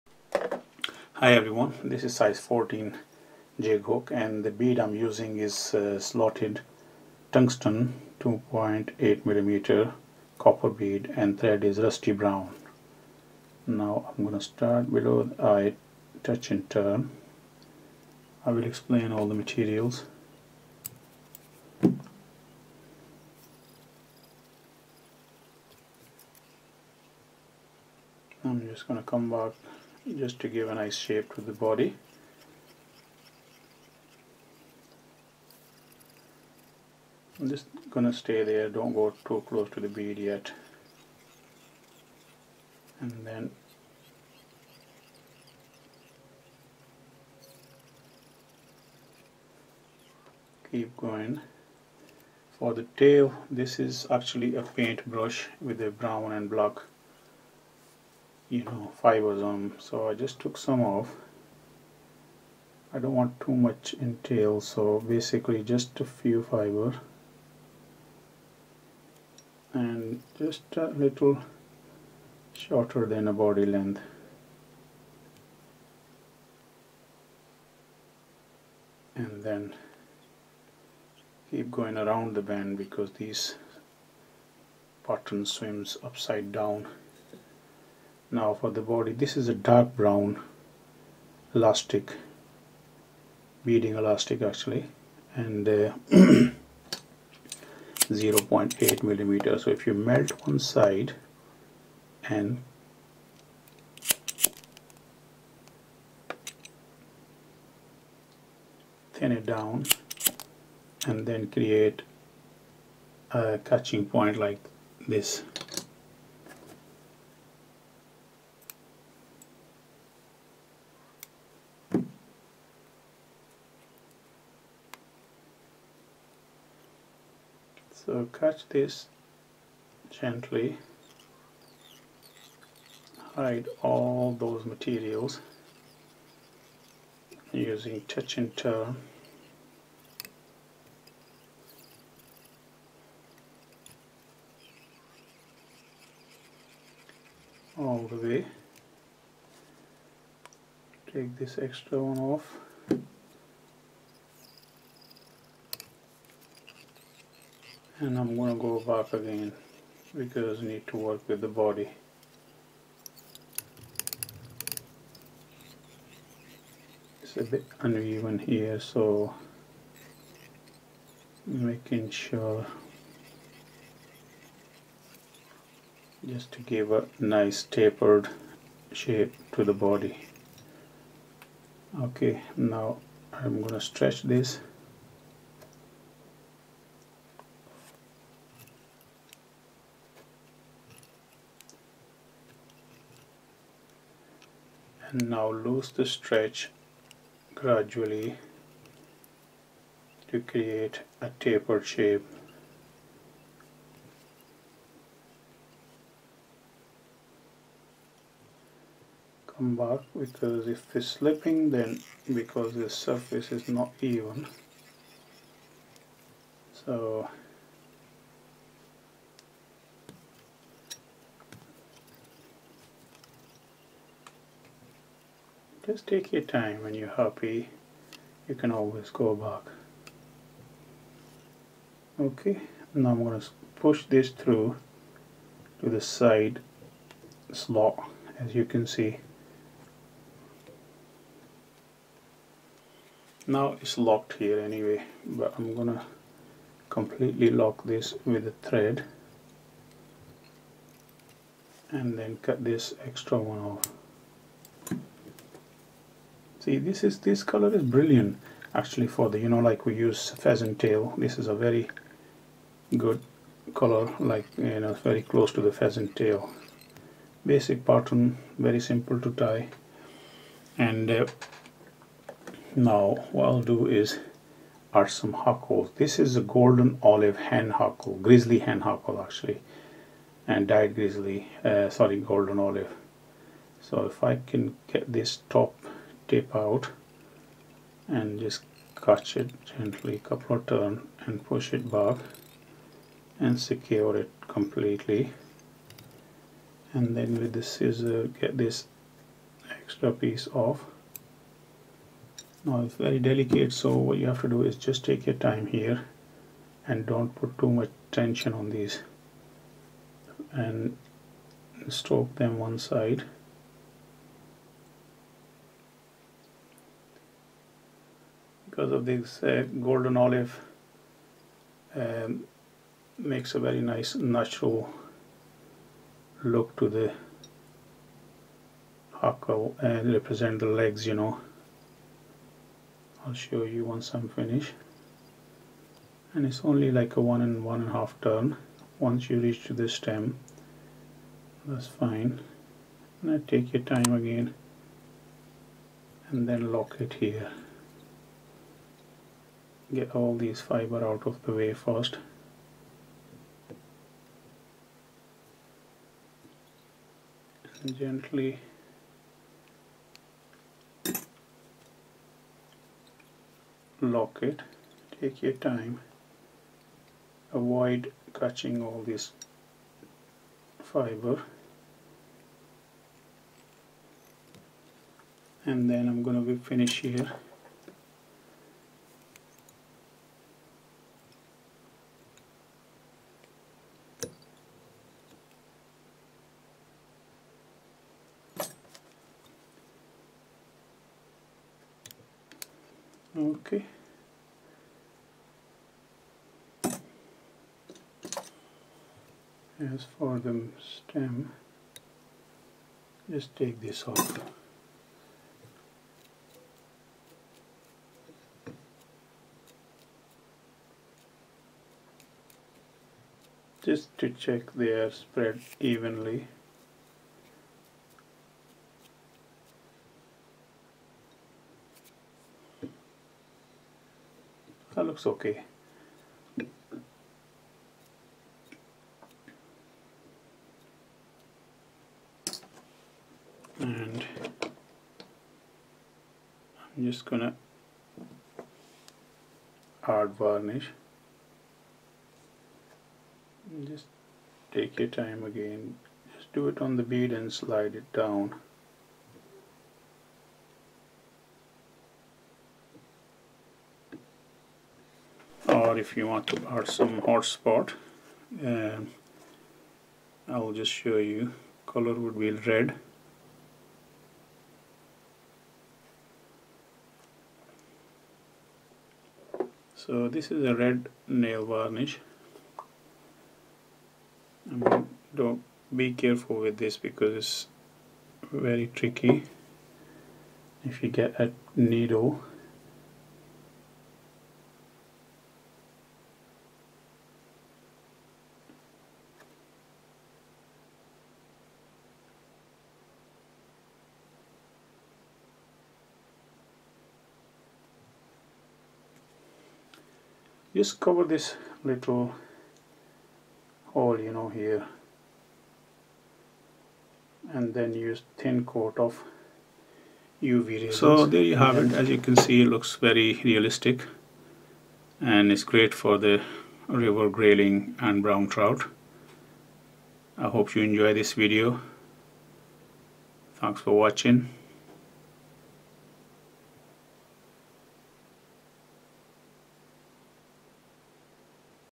Fly Tying an Effective Stretchy Jig by Mak
I really like this Pattern, for me it works really well for all Season,

Hook size12-14 ,, Thread Rusty Brown ,, Tail mix Red & Black Cock Hackle
Body Brown Stretchy Elastic ,, Hackles Grizzly Hen Dyed Ginger
Bead Tungsten Slotted